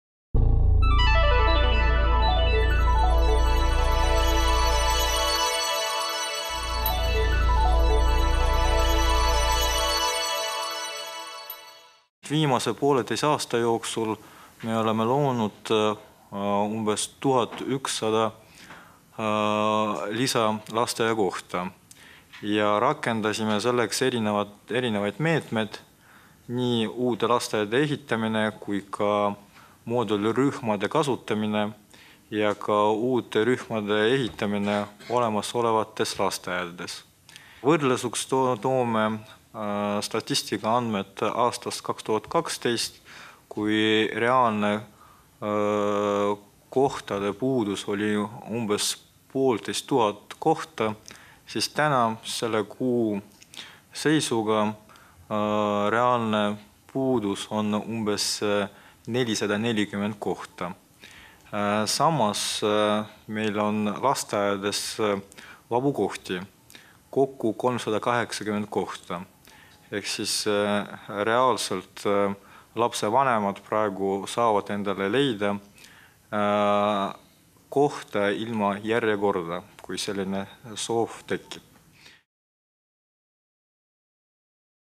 Pressikonverents 2014.10.08
Mihhail Kõlvart: Tallinna linna programmist "Lasteaiakoht igale lapsele".